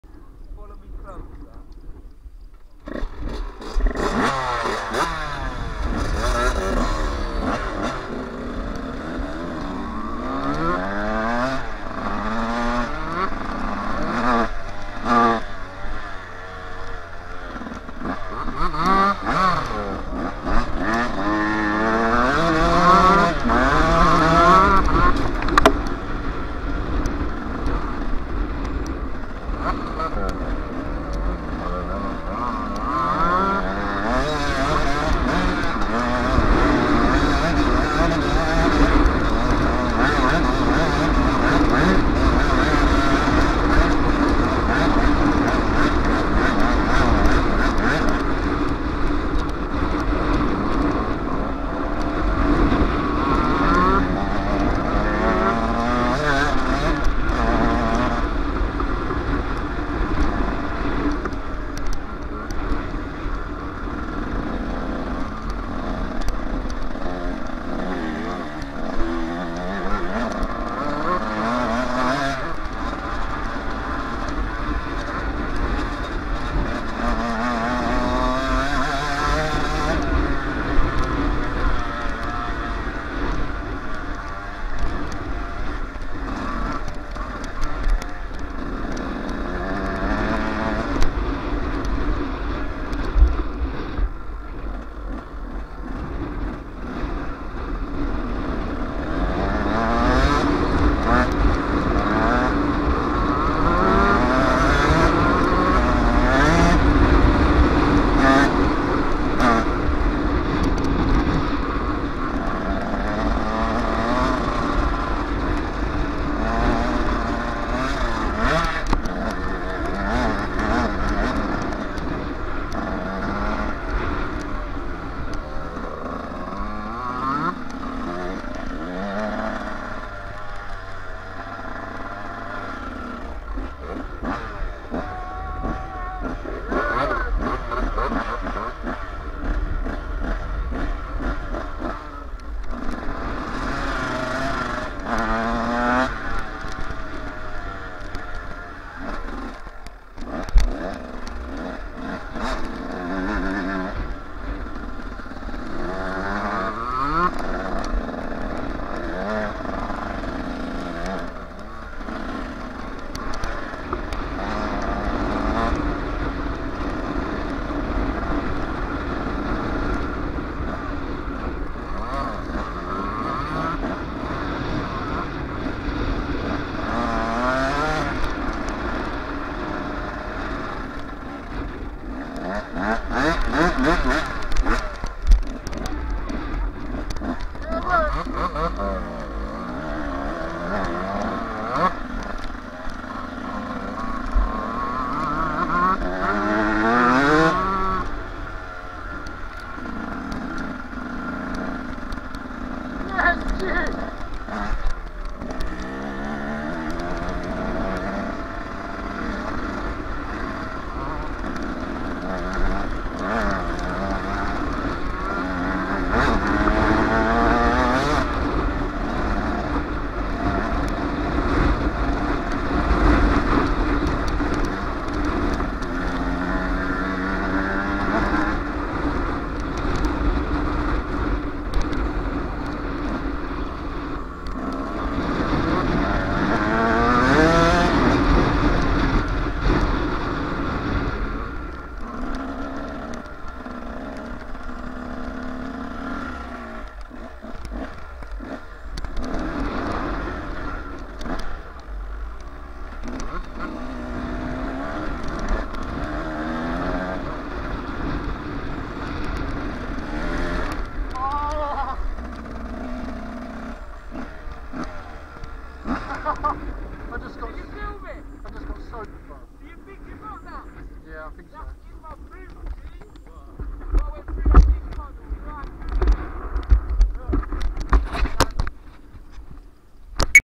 Out raceing round the quarry on the yamaha yz 125s
On my yamaha yz 125 1997 the other other is a 1998 model. Us raceing around the track!!